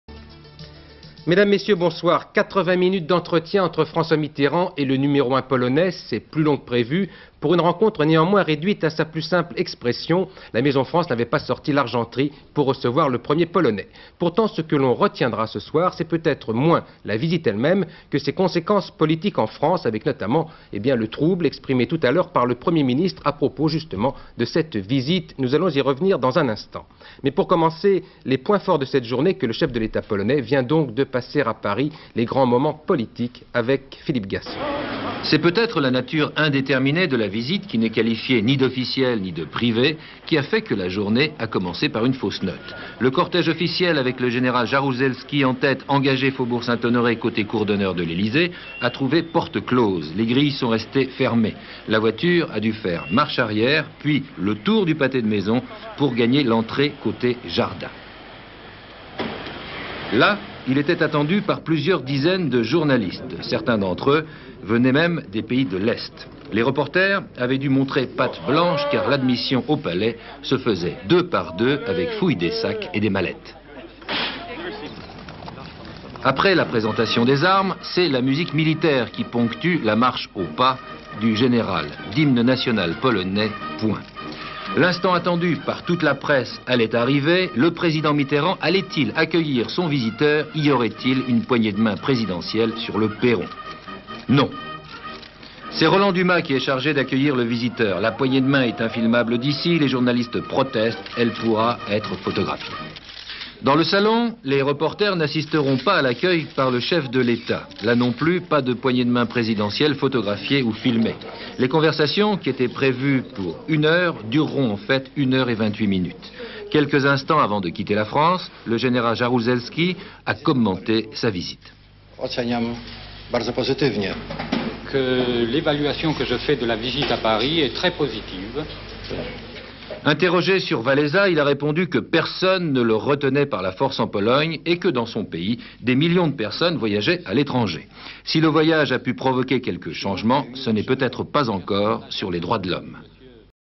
Le Général Jaruzelski à l'Elysée en 1985 - Archive INA
4 decembre 1985
Ce matin, le général Jaruzelski a été reçu pendant 90 mn par François Mitterrand, à l'Elysée. - Plan rapproché silhouette de Jaruzelsdi dan une voiture- Plan d'ensemble des grilles de l'Elysée fermées coté Faubourg Saint-honoré- Plan rapproché de la voiture qui fait marche arrière pour arriver par l'entrée côptée jardin-Divers plans des journalistes et des reporters attendant le général- Plan d'ensemble du général Jaruzelski marchant dans les jardins de l'Elysée/accueil sur le perron par Roland Dumas- Plan d'ensemble du général Jaruzelski et de François Mitterrand dans un salon de l'Elysée-Plan rapproché du général Jaruzelski commentant sa visite en polonais (traducteur officiant auprès de lui) : "visite très positive" politique; archive television; archive tv; ina; inna; Institut National de l'Audiovisuel; french tv Images d'archive INA